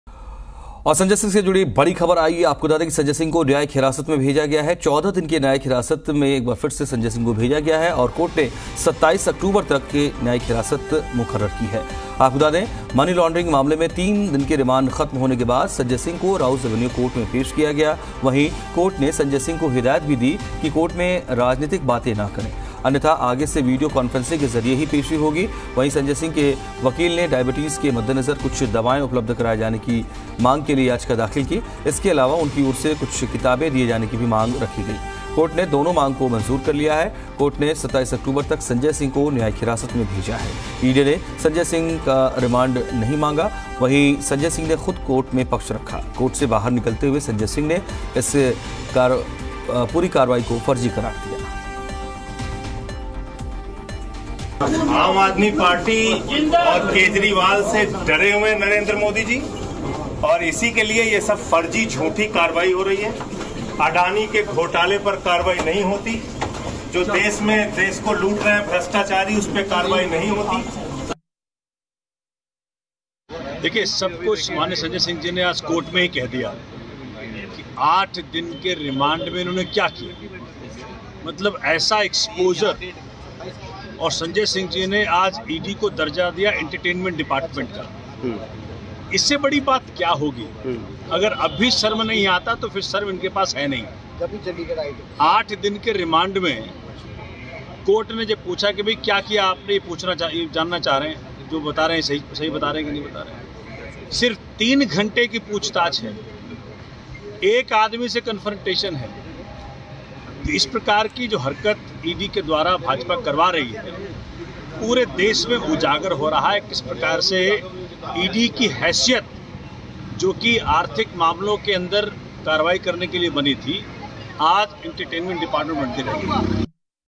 Sanjay Singh News - संजय सिंह को हिरासत में भेजा गया | 27 अक्टूबर तक न्यायिक हिरासत में भेजा
संजय सिंह को हिरासत में भेजा गया
27 अक्टूबर तक न्यायिक हिरासत में भेजा